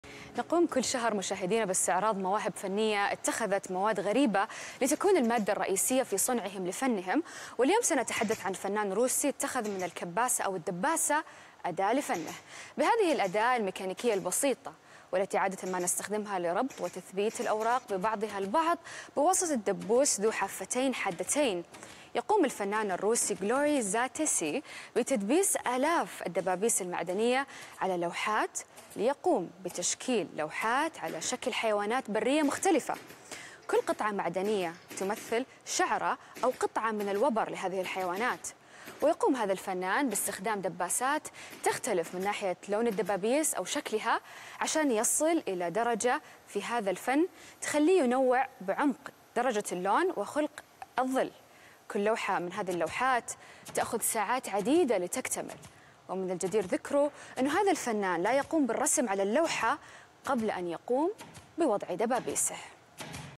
في فقرة الثقافة والفنون.. فنان روسي يرسم لوحاته معتمدا على الدباسة كأداة رئيسية.. وآلاف الدبابيس!
في فقرة الثقافة والفنون..
فنان روسي يرسم لوحاته معتمدا على الدباسة كأداة رئيسية.. وآلاف الدبابيس!